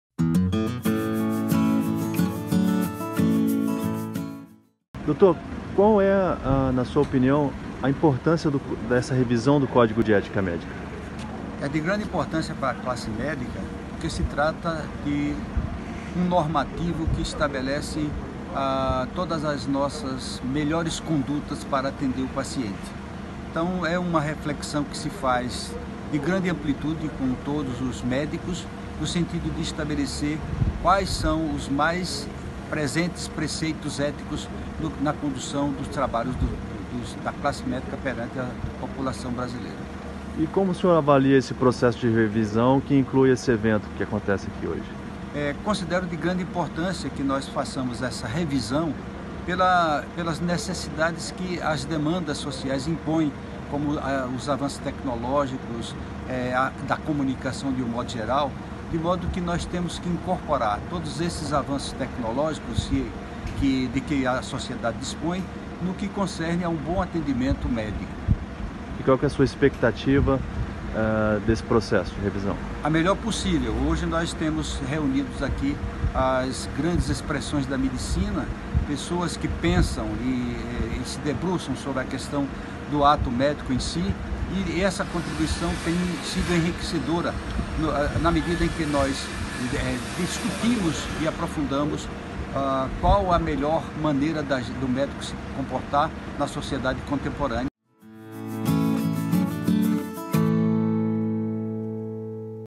Atualização CEM - Henrique Batista e Silva
Entrevista com Henrique Batista e Silva, Secretário-geral do CFM.